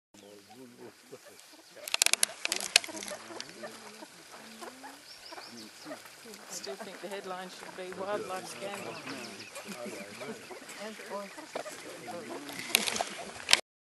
Crested Francolin
The Crested Francolin (Dendroperdix sephaena) is a species of bird in the Phasianidae family.
This particular bird decided to take a close look at the camera!